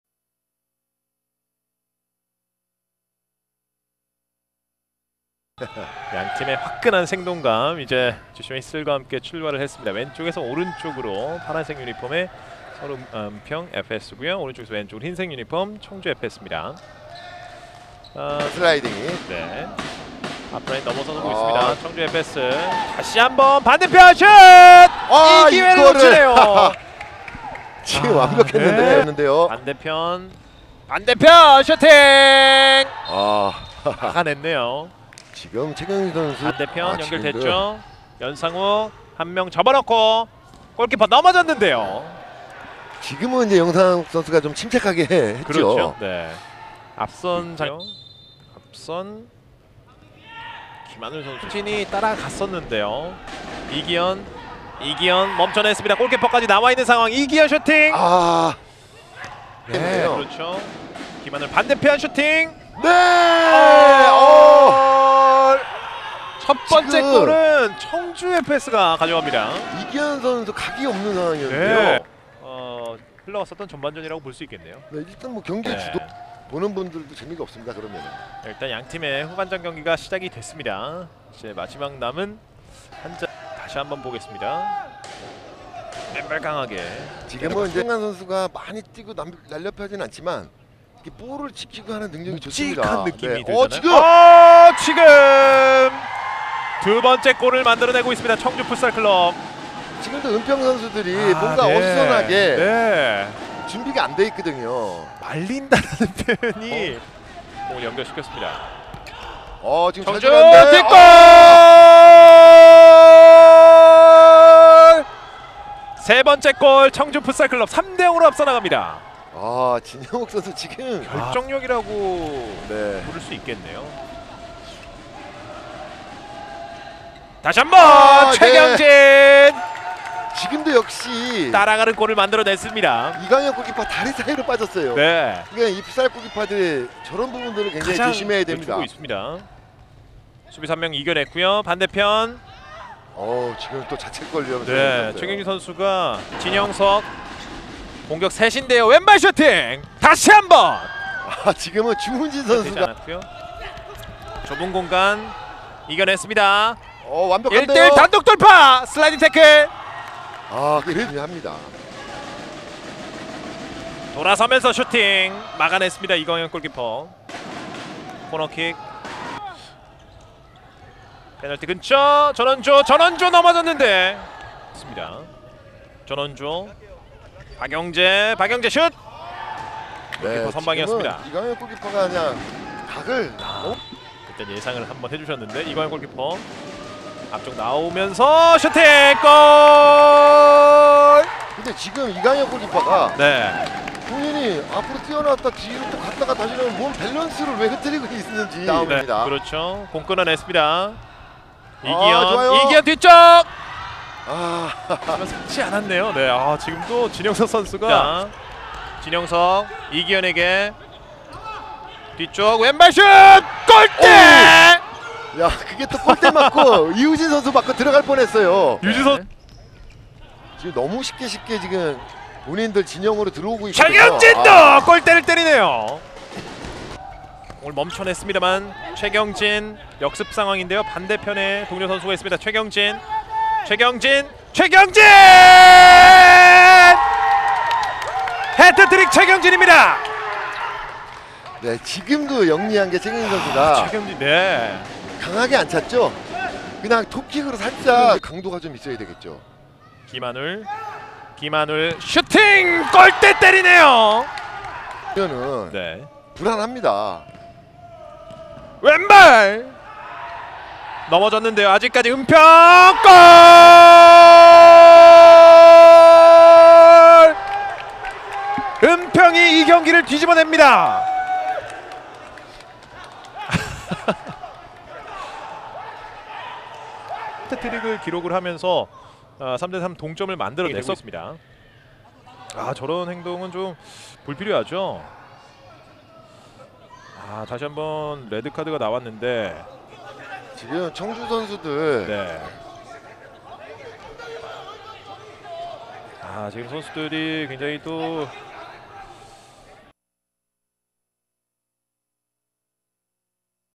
서울은평FS vs 청주풋살클럽 하이라이트 : 현대해상 2018 FK CUP 2018 7 8 횡성국민체육센터
2018.7.8
횡성국민체육센터
현대해상 2018 FK CUP
서울은평FS vs 청주풋살클럽